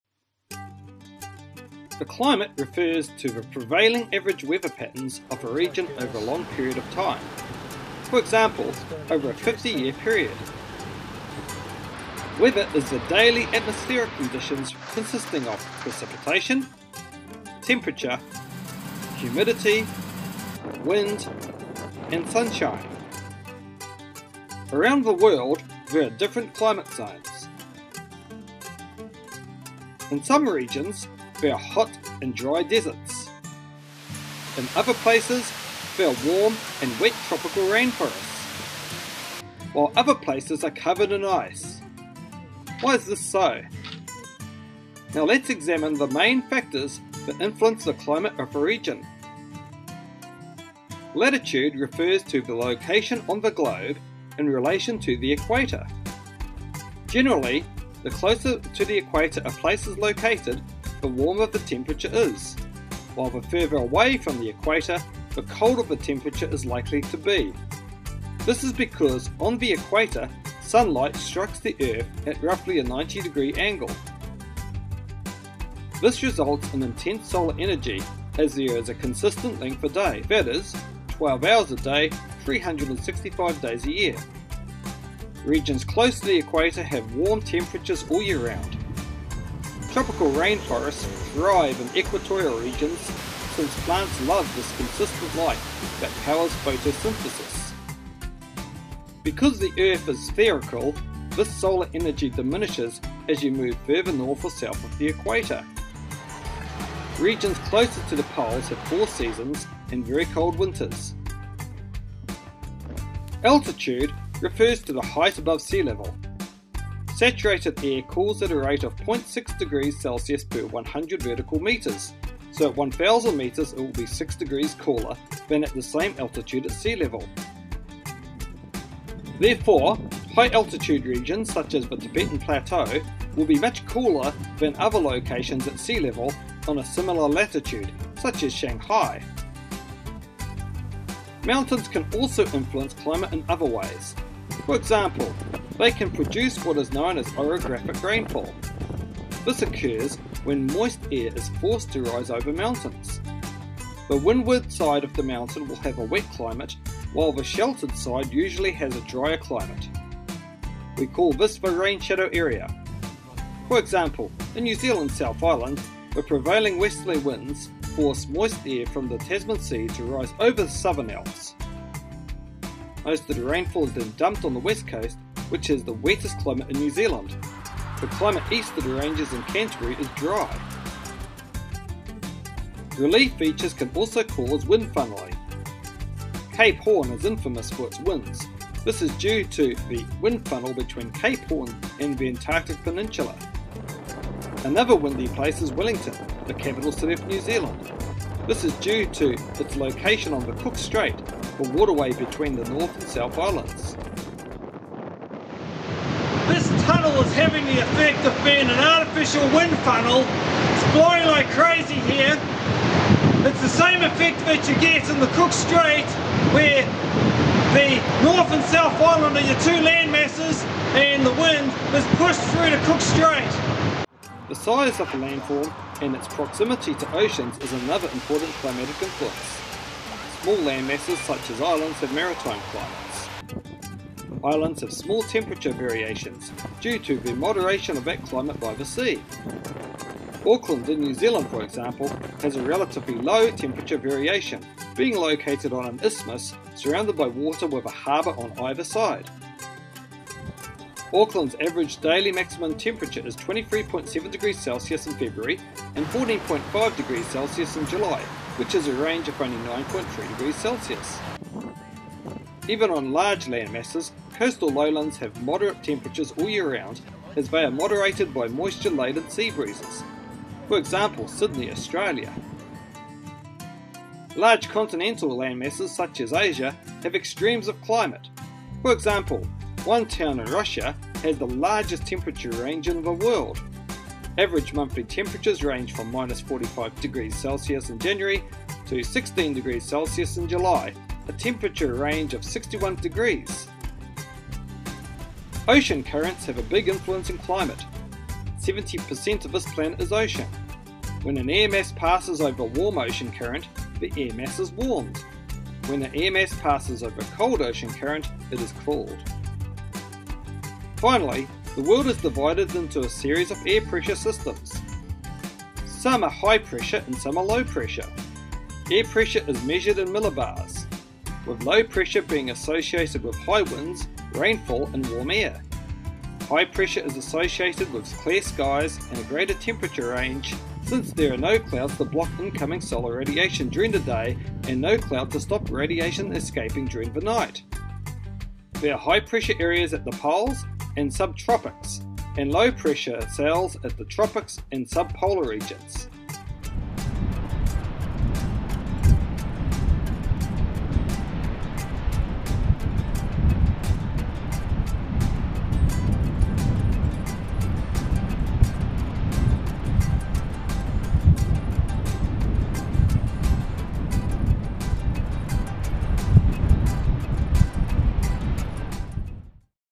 Why does Climate vary around the world?
Global Climate is affected by factors known as Climate Controls:
1. Latitude
2. Mountains.
3. Ocean Currents
4. Air pressure systems.
5. Proximity to oceans.
 6. Size of a landform.